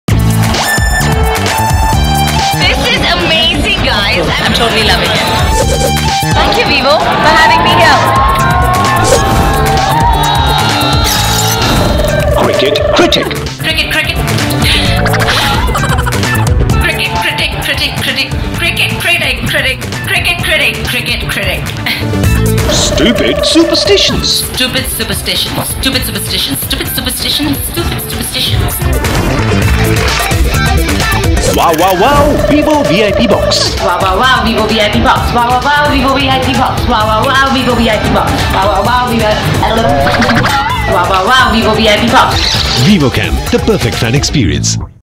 Vivo Cam - Archana Vijaya
The gorgeous Archana Vijaya had a great time at the VivoCam, trying her best to get the tongue twisters right! How many did you get correct?